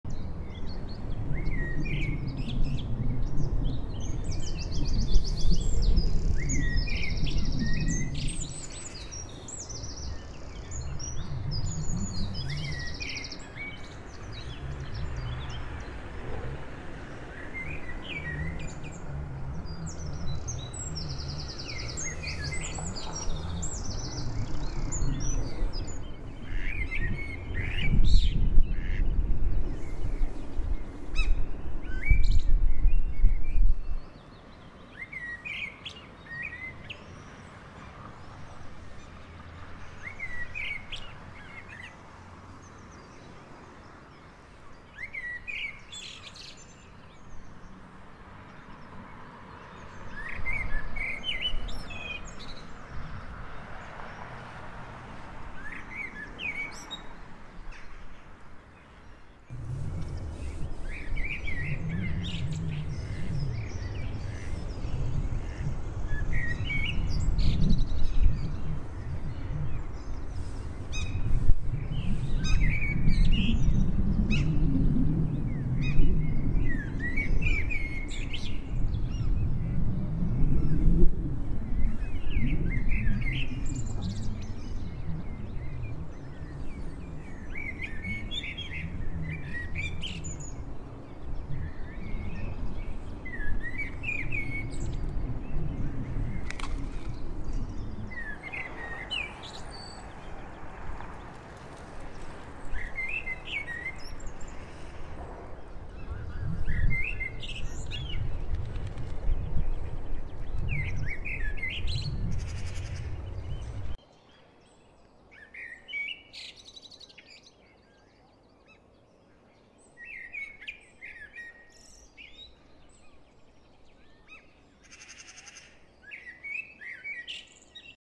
Jackdaws collecting horse hair as nesting material
Eurasian Jackdaws (Corvus monedula) collecting horse hair as nesting material. In Heemskerk, The Netherlands. 4K